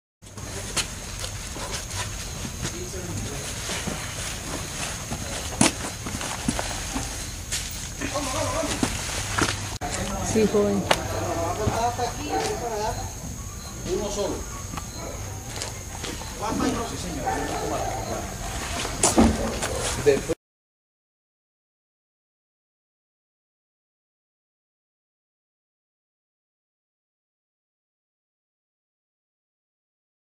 Armada incauta cocaína en cercanías al mercado de Bazurto
La Armada Nacional decomisó 28 kilogramos de cocaína cuando eran transportados en un camión de estacas en el sector del mercado de Bazurto en Cartagena.